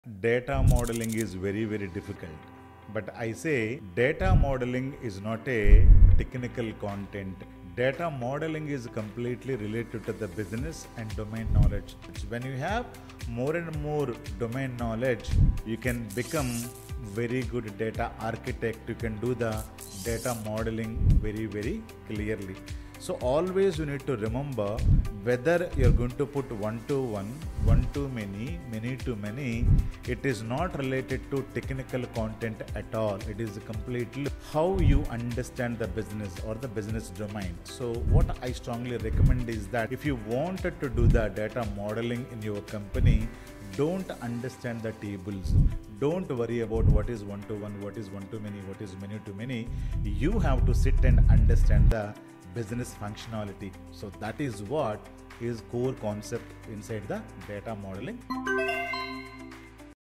Data modelling is not just a technical concept. It is all about business knowledge.
Many people think data modeling is a tough technical task...

But here’s the truth

It’s NOT about tables, SQL, or code.
It’s ALL about understanding the business and domain knowledge.

Want to become a great Data Architect?
Focus less on ER diagrams and more on real-world business functionality.
✅ Know how the business operates
✅ Understand what connects to what and why
✅ THEN decide on One-to-One, One-to-Many, or Many-to-Many

Don’t start with tables.
✅ Start with conversations with stakeholders.

Master the business → Model the data clearly.